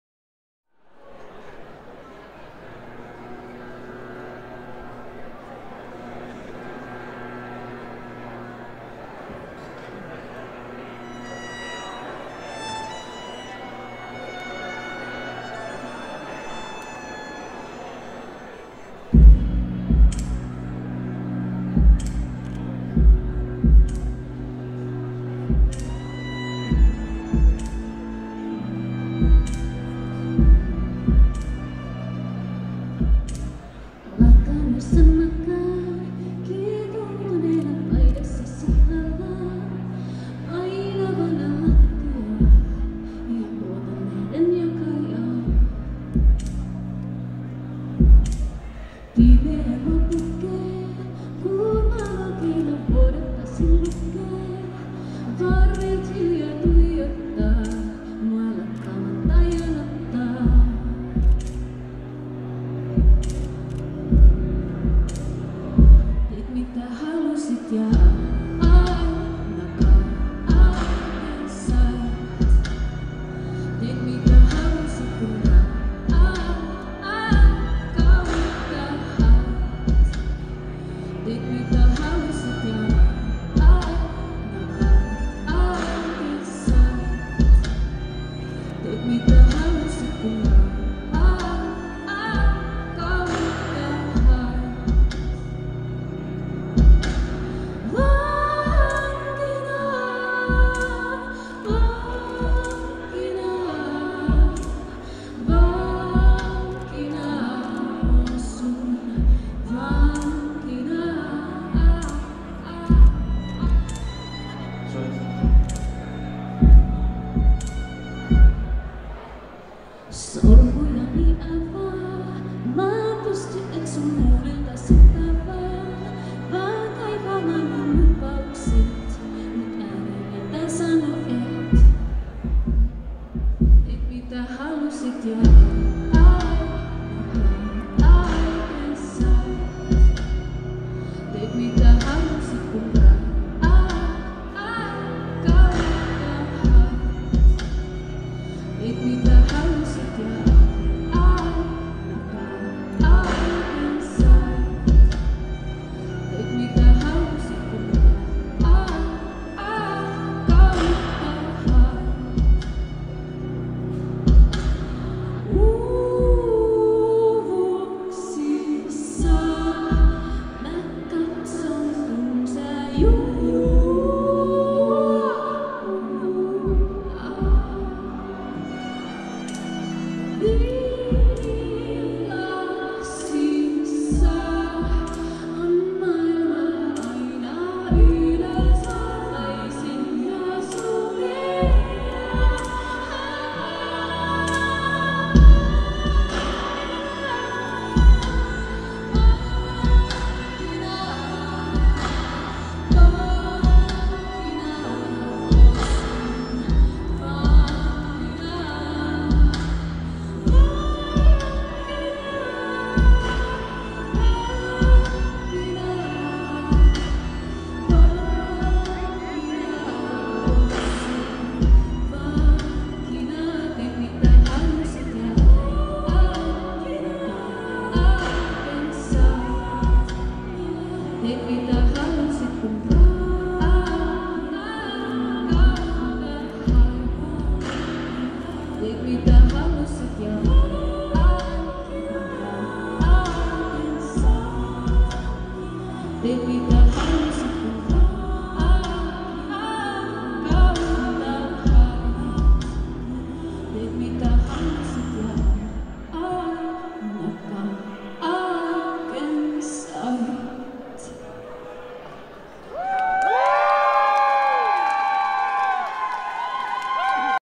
Aino Elina @ ABC Glasgow Scotland 10/2/2018
SoundWaves Music Competition featuring The Smokin' Bugler Band, Emmanuelle Gabarda, Aino Elina, Bitterwood, Glass Ships, Gentlemen Jackals, Xiles, Cara Tavendale, Kingsmen, Silver Coast, Dominicide, Danny Mortimer, Eriska, Stuffed Animals